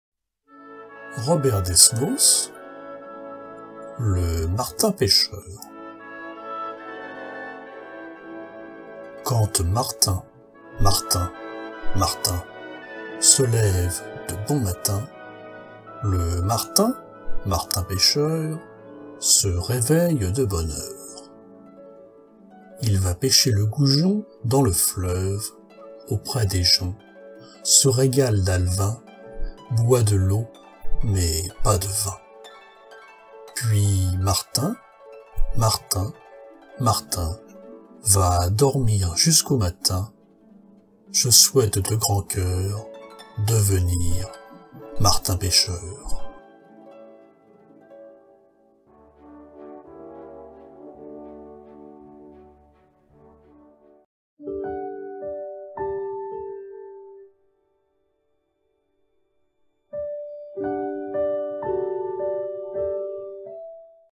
Le Martin-pêcheur - Chantefables - Robert Desnos
Le Martin-pêcheur
Chantefables et Chantefleurs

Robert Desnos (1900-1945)

Lecture : AB
Portrait de couverture : Raphaël Gauthey
Illustration de fond : Paul Klee

Texte :
Quand martin, martin, martin
Se lève de bon matin,
Le martin, martin-pêcheur
Se réveille de bonne heure.

Il va pêcher le goujon
Dans le fleuve, auprès des joncs,
Se régale d’alevins,
Boit de l’eau mais pas de vin.

Puis martin, martin, martin
Va dormir jusqu’au matin.
Je souhaite de grand cœur
Devenir martin-pêcheur.